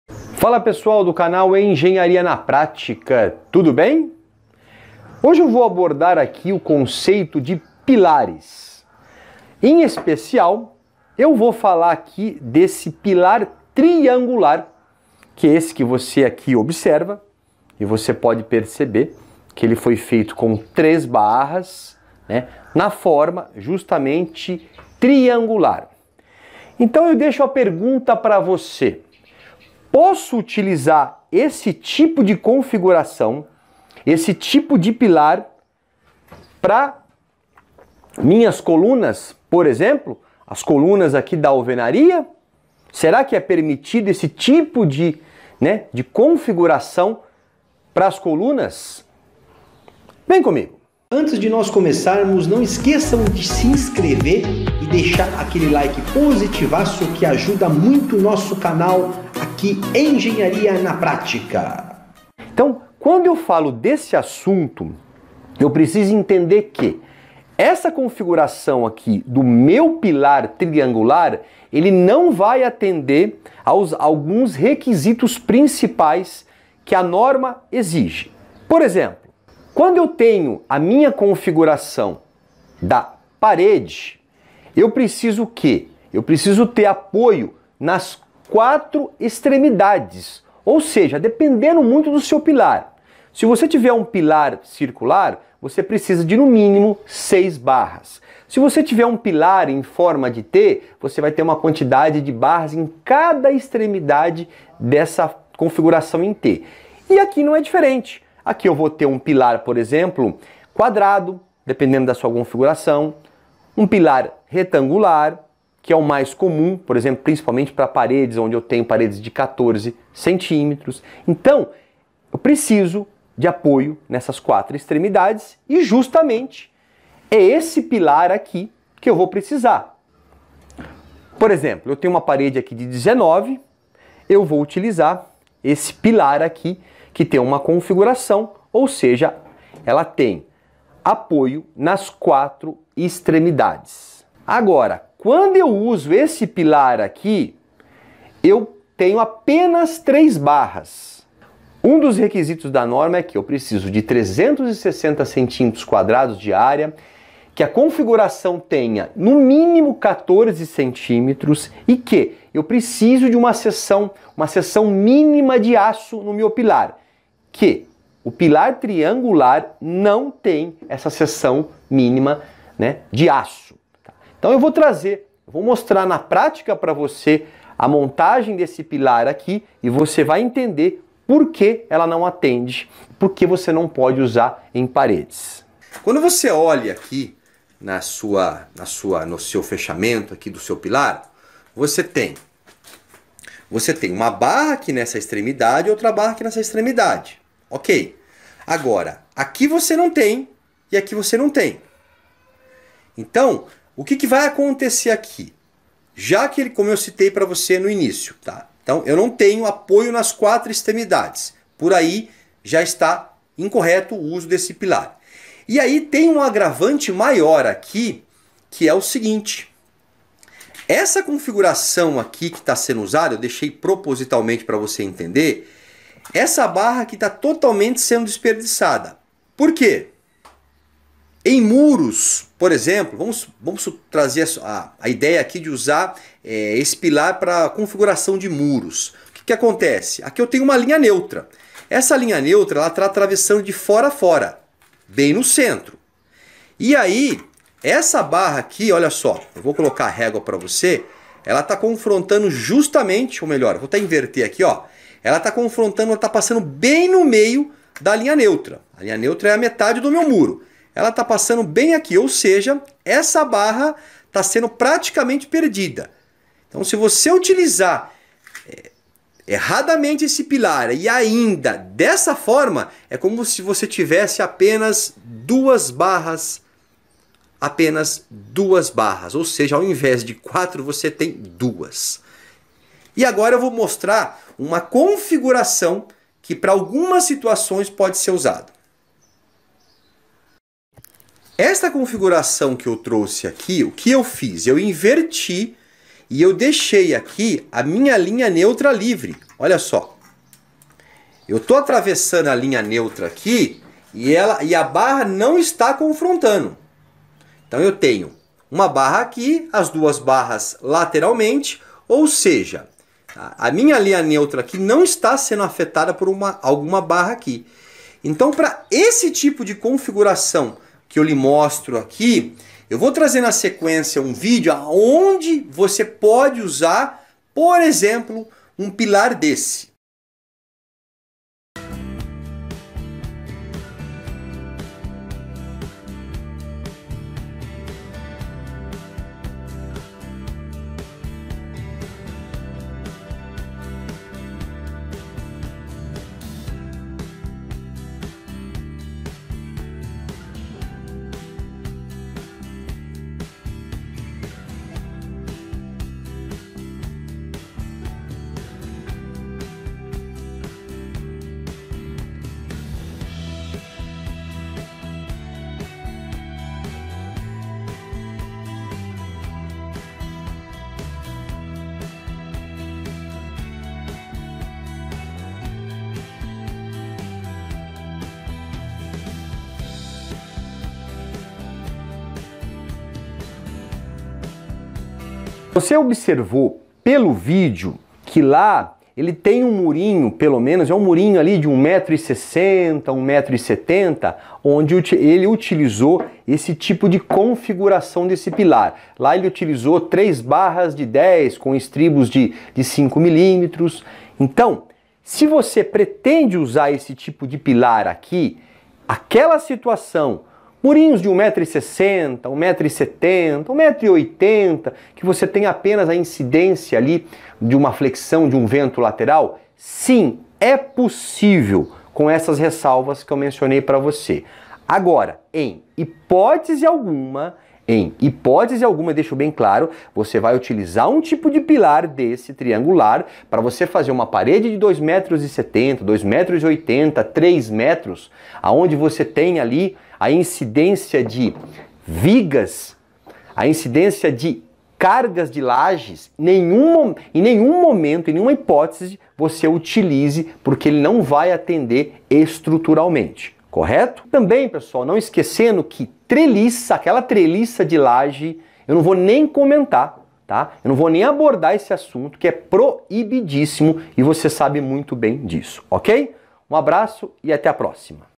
PILAR COM TRELIÇA PIDE SER USADO?
Este vídeo mostra o conceito do uso de PILARES TRIANGULARES...
Possível?
De que forma?
Será?